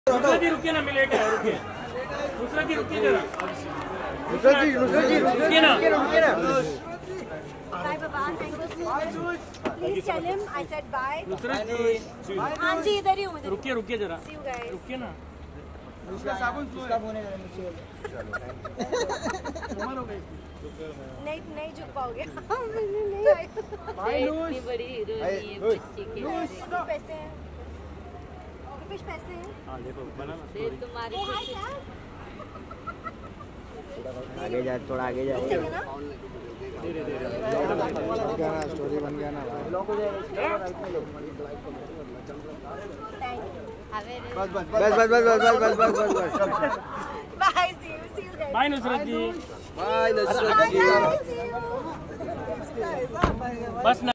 NUSHRRATT BHARUCCHA SPOTTED AT LOS CAVOS RESTAURANT IN BANDRA
Description:
"🌟 Nushrratt Bharuccha was recently seen at the vibrant Los Cabos Restaurant in Bandra, and we've got the exclusive scoop for you! 📸 Dive into this celebrity sighting as Nushrratt indulges in exquisite cuisine and shares a glimpse of her day. Don't miss out on this exciting glimpse into the star's life! ✨"